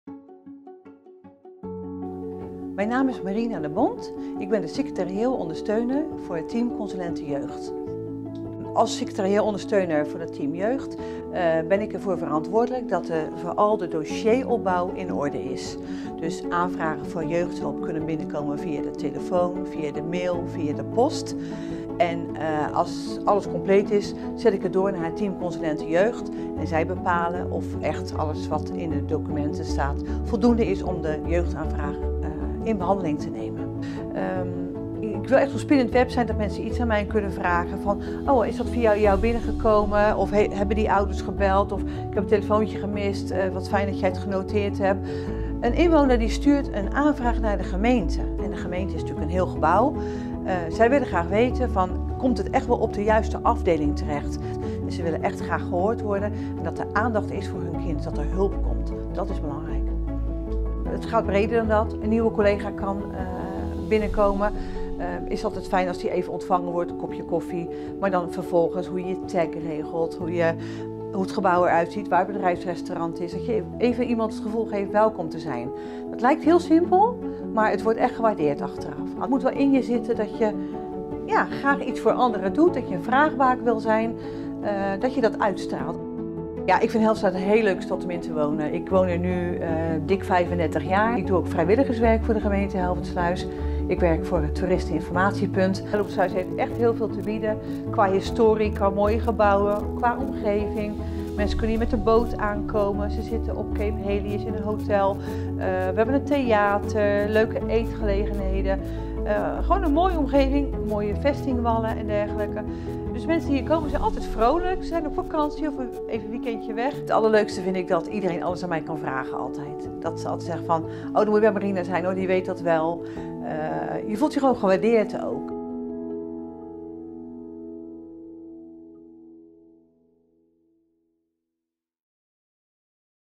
Werken voor Hellevoetsluis -Marina
Werken in de administratieve ondersteuning bij een gemeente betekent flexibel zijn, goed en duidelijk kunnen communiceren en overzicht kunnen houden. Marina ziet het als een uitdaging om een optimale dienstverlening aan de klanten te realiseren. Mooi werk op een mooie plek!